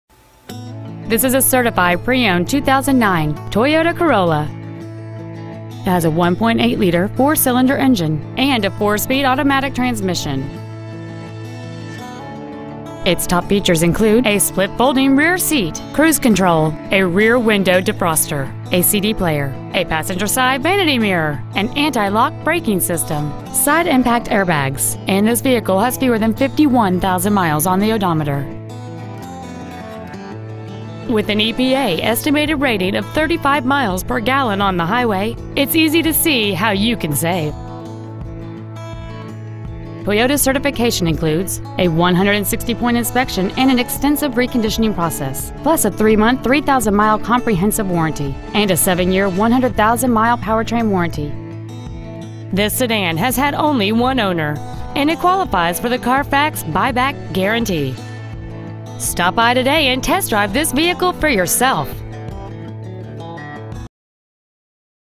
Certified 2009 Toyota Corolla Concord CA 94520
Year : 2009
 Make : Toyota
 Model : Corolla
 Engine : 1.8L I-4 cyl
 Trans . : 4-Speed Automatic
 Exterior : Silver
 Miles : 50,902
 Interior : Black
 Concord Toyota
 2009 Toyota Corolla Concord CA